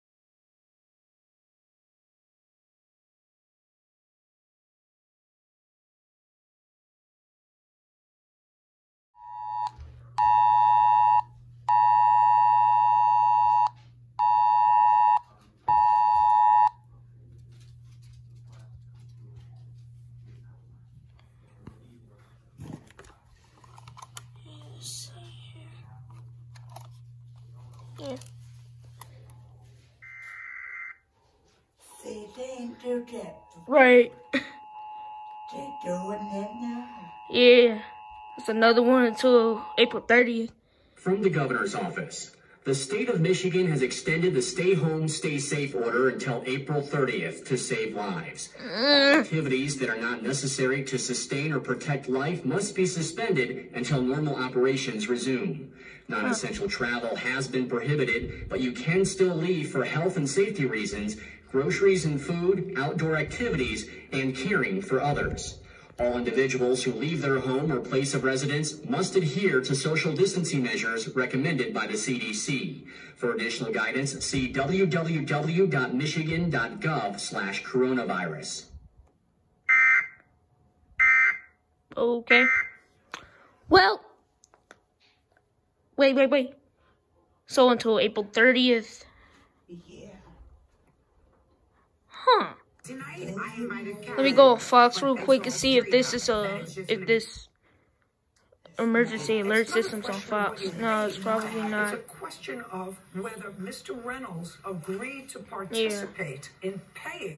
State Of Michigan EXTENDED Shelter In-Place Order For Coronavirus- Civil Emergency Message (EAS 113)
This was recorded today on 4/9/20